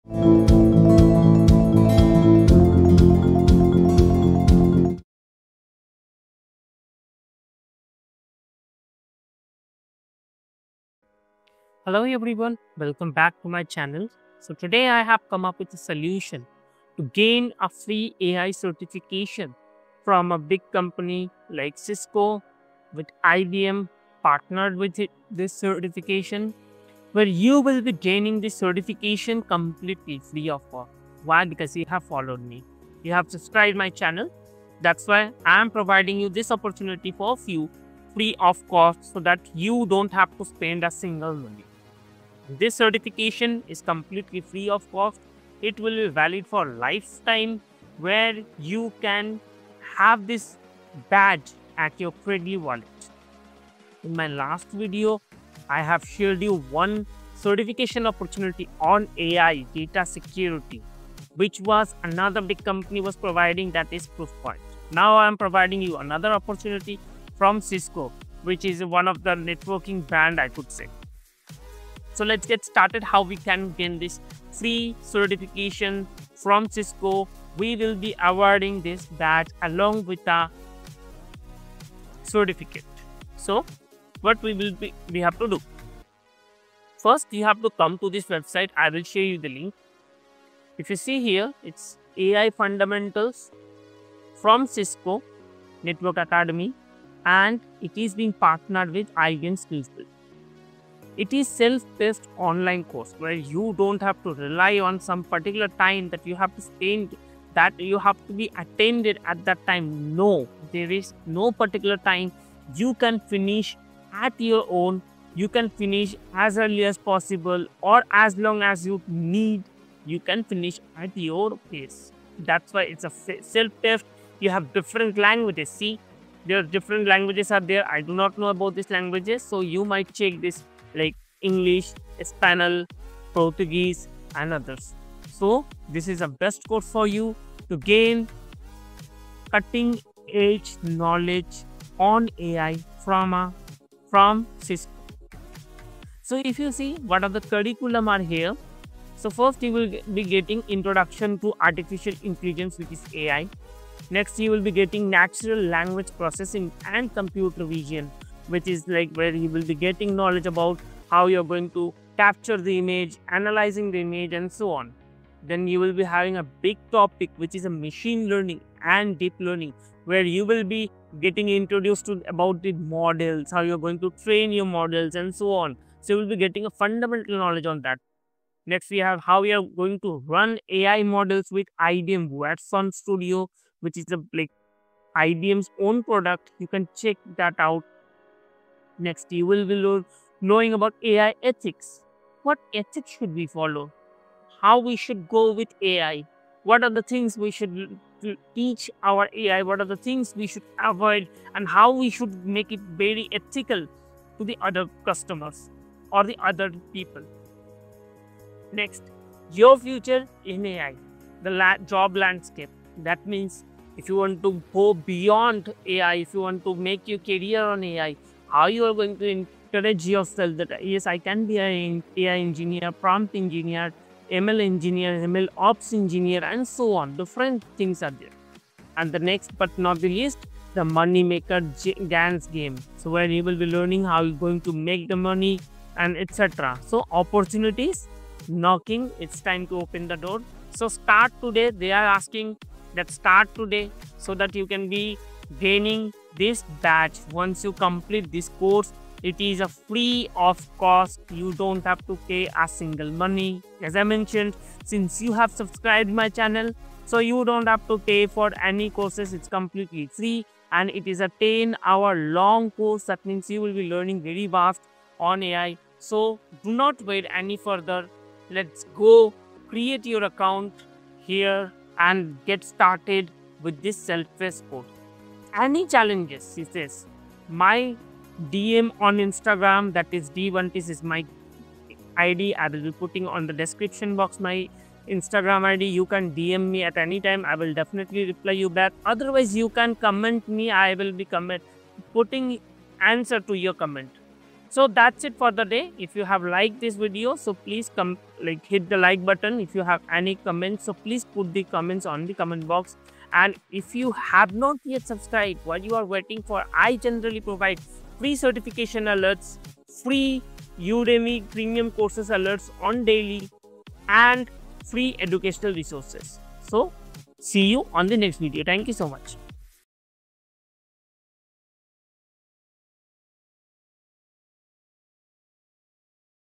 Unlock 2025: Your Guide to 100% FREE AI Certifications
Cisco, in collaboration with IBM SkillsBuild, offers the AI Fundamentals with IBM SkillsBuild course—verified with an official badge. By completing this free program, you’ll gain student-level credentials and hands-on knowledge of key AI concepts like Natural Language Processing, Computer Vision, Deep Learning, and AI Ethics.

This course is designed to help you:

* Understand the history and evolution of AI
* Explore how AI interprets language and images
* Train and test machine learning models using IBM Watson Studio
* Learn the role of ethics in AI
* Prepare for real-world AI jobs and careers
* Even experiment with the exciting world of GANs (Generative Adversarial Networks)

🌍 Why this matters: AI is reshaping the future of work, and certified knowledge can make your resume and LinkedIn stand out instantly. The best part? You don’t need to spend thousands—this opportunity is completely free.

📌 What you’ll learn in this guide:

* Natural Language Processing & Computer Vision
* Machine Learning & Deep Learning
* Running AI Models with IBM Watson Studio
* AI Ethics & Responsible Use
* The AI Job Landscape in 2025
* The Money Maker: GANs Game

✨ Opportunity is knocking—it’s time to open the door. Start today and take the first step toward becoming an integral part of the AI-driven workforce of tomorrow.